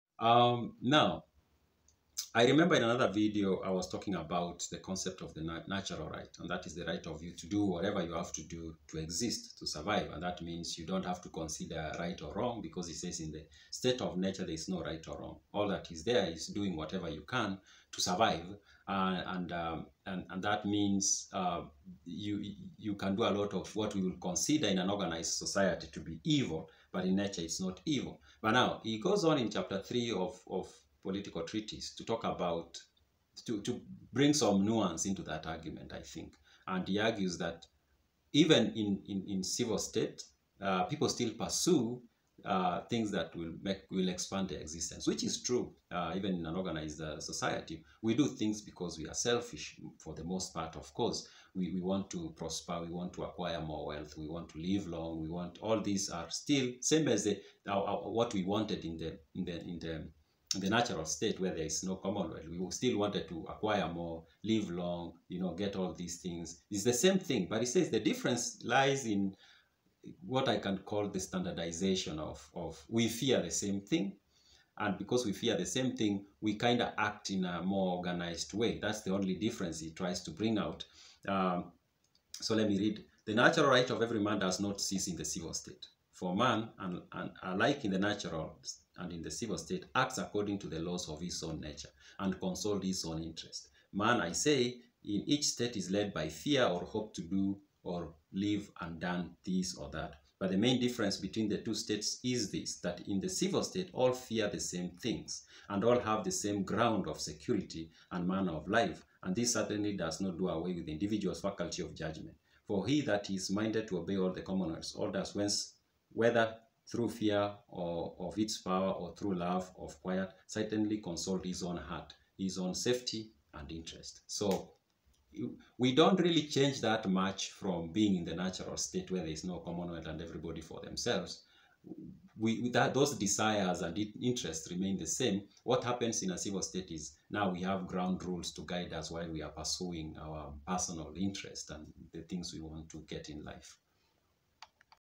Motivation To Act In Nature And In A Civil State Is Same But..| Political Treatise by Baruch Spinoza
In chapter three of Political Treatise, Baruch Spinoza tells us that in both nature and in a civil state individuals are motivated by selfish reasons in their actions. How there is one major difference in the two scenarios.